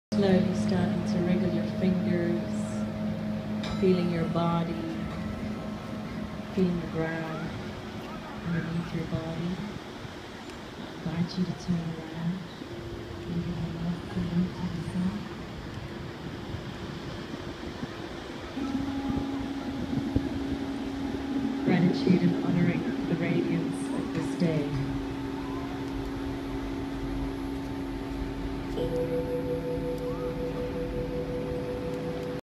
Meditation Session in Cape Town || ViralHog
Occurred October 27, 2015 / Camps Bay Beach, Cape Town, South Africa

Talk about a scenic spot for a meditation session!